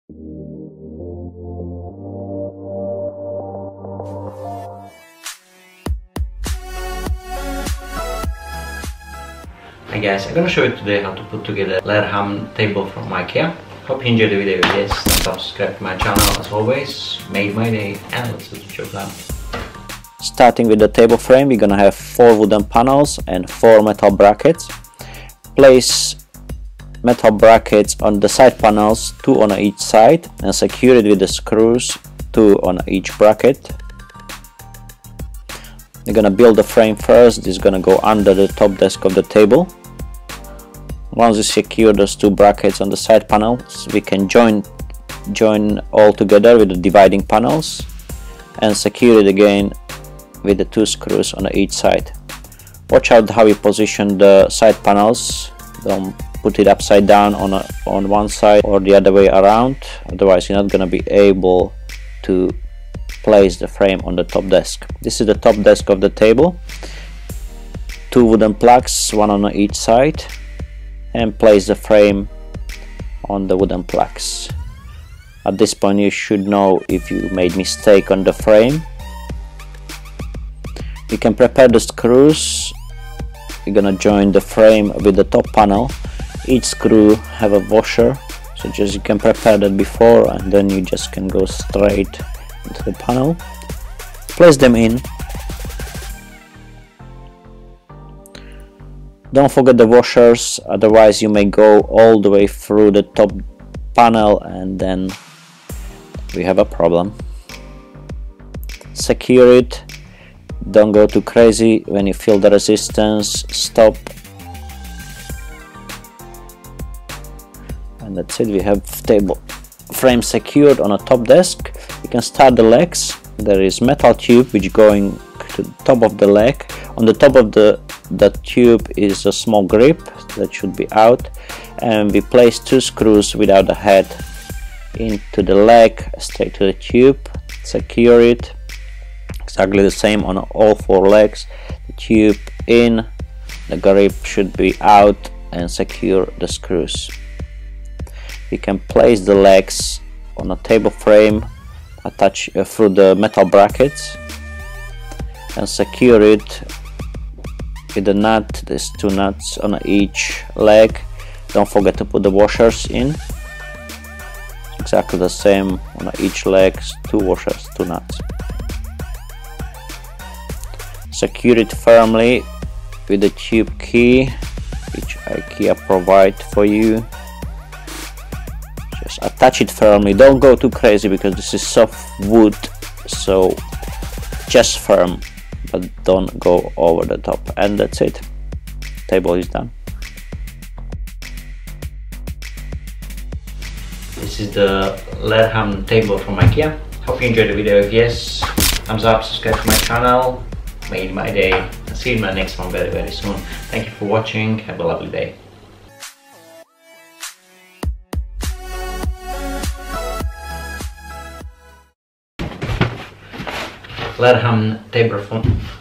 IKEA Table LERHAMN Assembly
How to assemble IKEA Table LERHAMN Light antique stain white stain there is an option for different size. This one in video LERHAMN Light antique stain ramna beige mesurements 74x74 cm.

*** TOOLS IM USING ***
Small electric screwdriver
Bigger drill and screwdriver all in one

Here is my stuff :
Flat Pack Furniture Assembly London